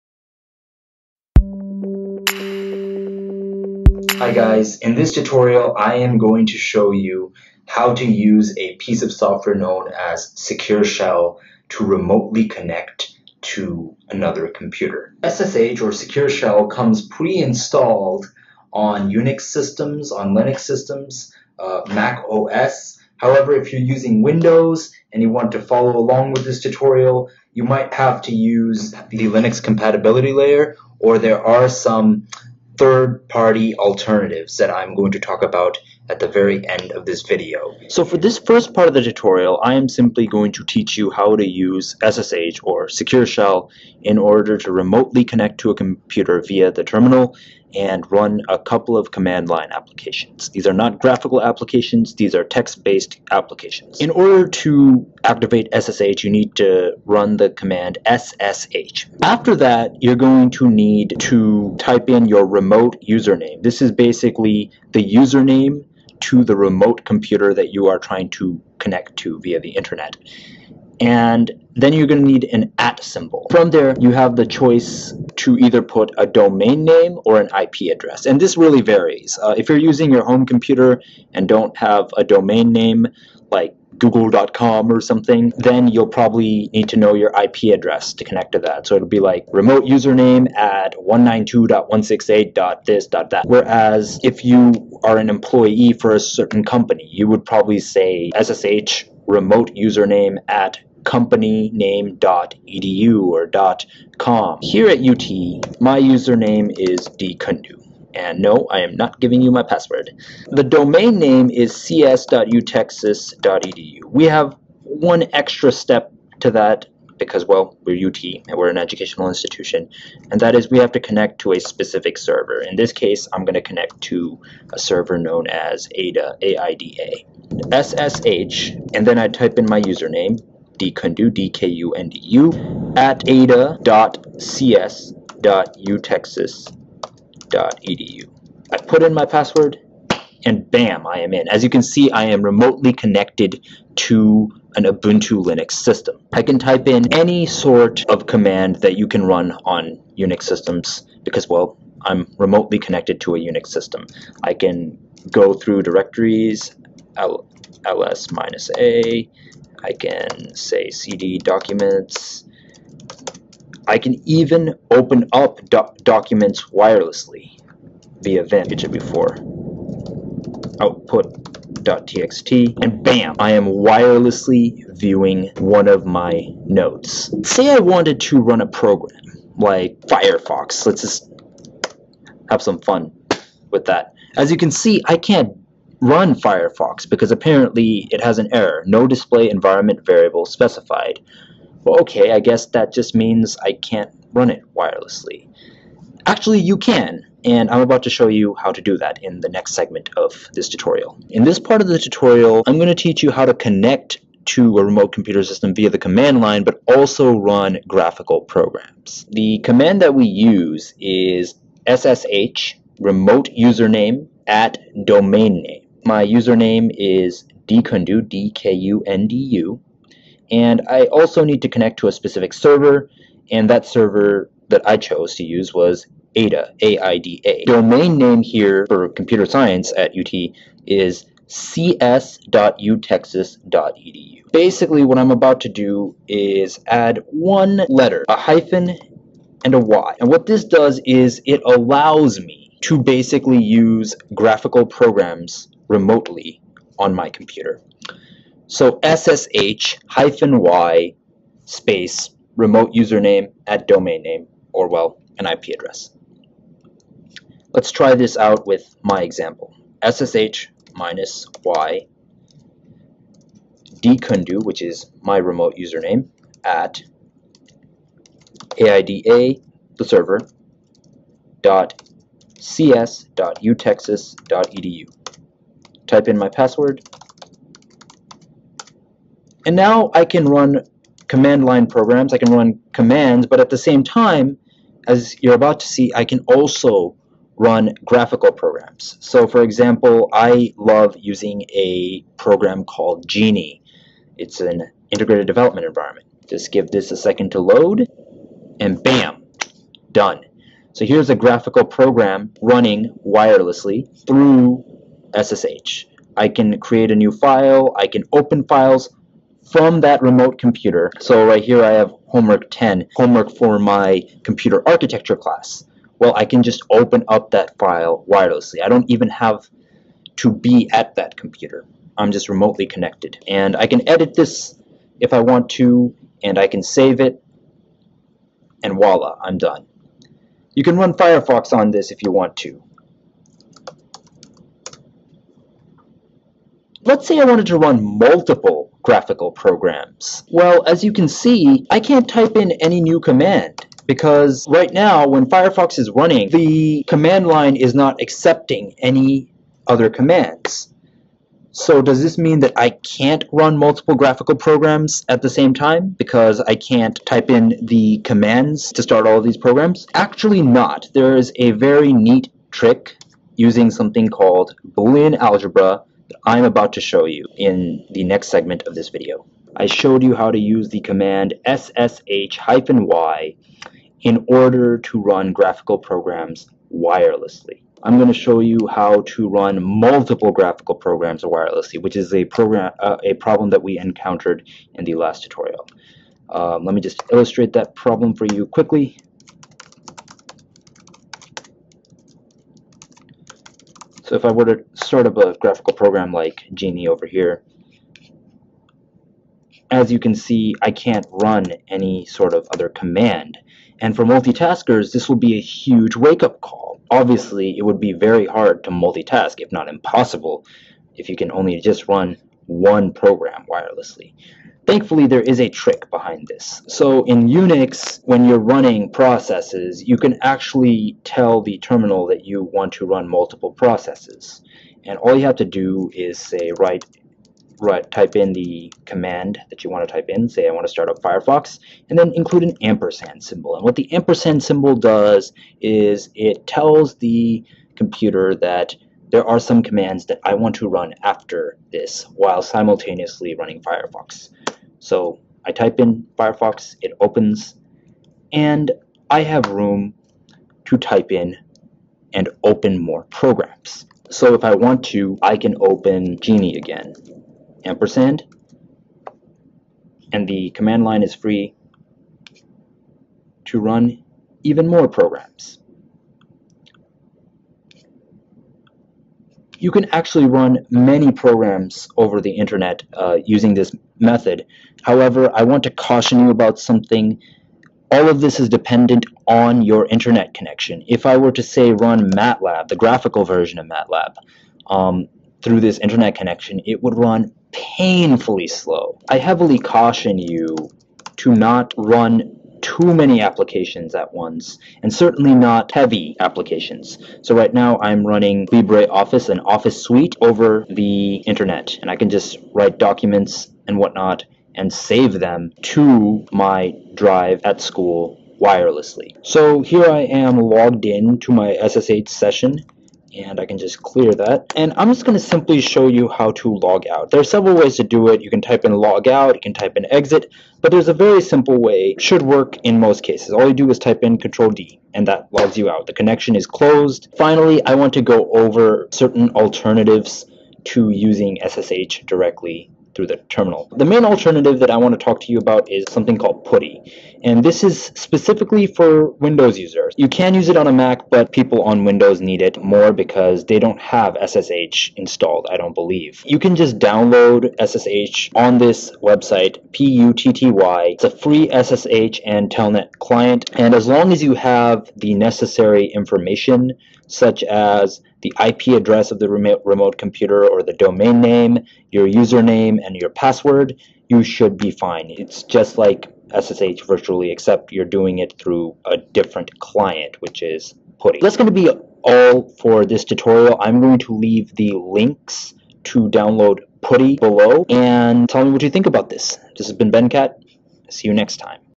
Secure Shell (SSH) Tutorial
Using SSH to login, run programs (both graphical and command line), and disconnect from remote computers.